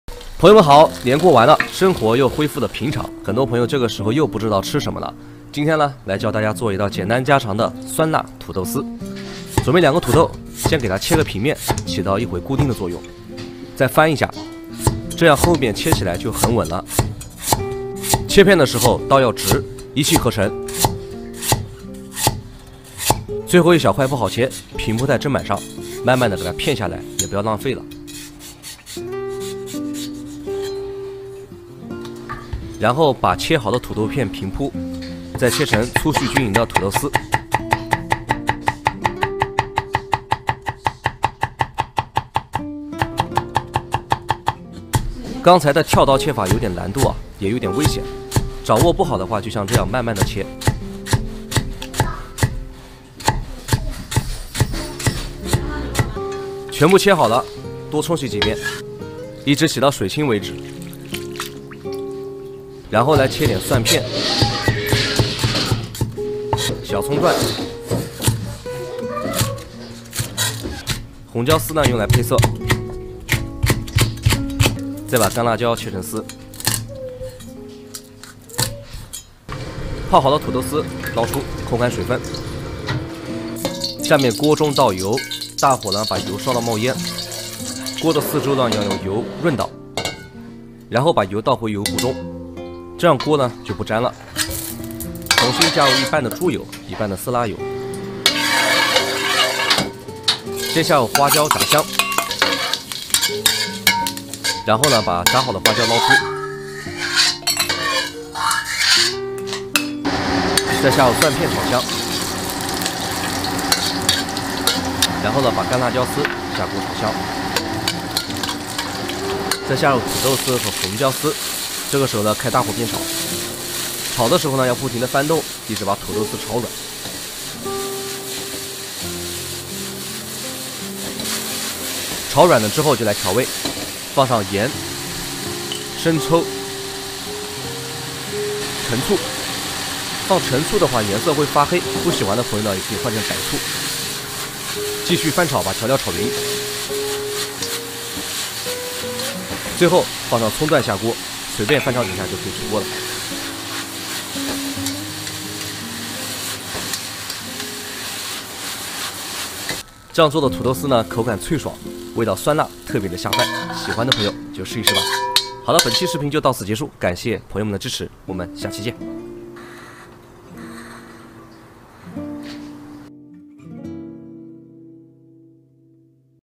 大厨教你酸辣土豆丝的正确做法，香辣酸爽又开胃，下酒又下饭！【美食强】
大家好，年过完了，生活又恢复了平常，今天强子来给大家分享一个家常菜酸辣土豆丝的正确做法，香辣酸爽又开胃，下酒又下饭~